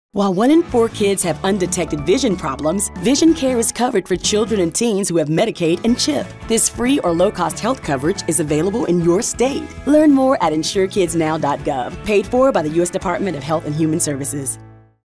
Video: “Vision Health” (English) — :15 seconds
Peace of mind is knowing your child has access to quality vision care. About 1 in 10 preschoolers and 25% of kids in grades K-6 have vision deficiencies. When vision problems go undetected in children, it can lead to impaired development or a misdiagnosed learning disability. Regular comprehensive eye exams are an important part of routine preventive care. Kids and teens up to age 19 are covered for vision care, including routine eye exams, eyeglass frames, and lenses under Medicaid and the Children’s Health Insurance Program (CHIP).

Families without health insurance may be eligible for free or low-cost coverage available through Medicaid or CHIP and they can enroll at any time of the year. Parents may be eligible for Medicaid, too.